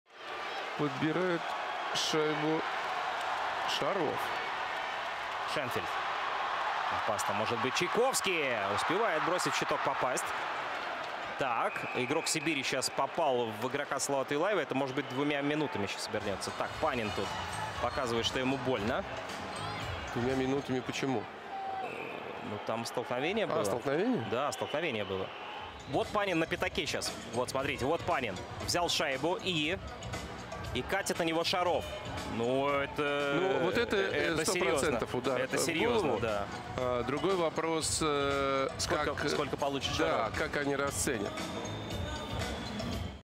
Sharov gets game-misconduct for checking Panin
Sharov gets game-misconduct for checking Panin to the head illegally

Александр Шаров получает 5+20 за атаку Григорию Панина в голову